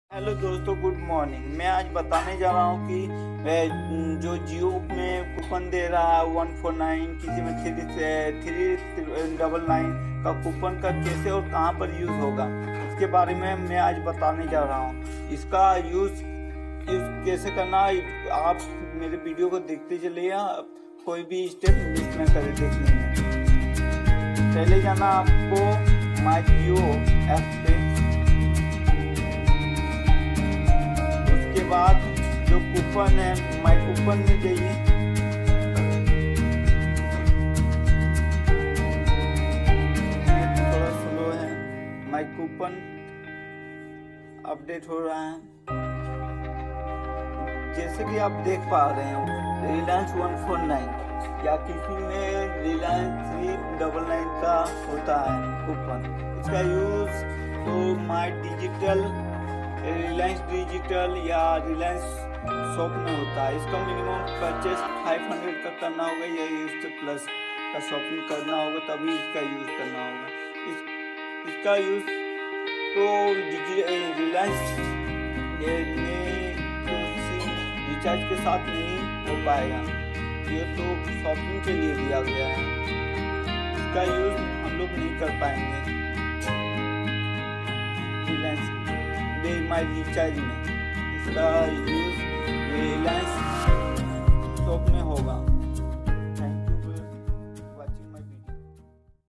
How to use Reliance 149 ya 399 coupon Apply .....
Mai ajj batane Ja rha hu ki Reliance ka coupon apply karna h.
Bhai log ajj ka video bahut hi special h .kyu ki ajj mai batana chahta hu ki Reliance ka coupon kese apply karna h .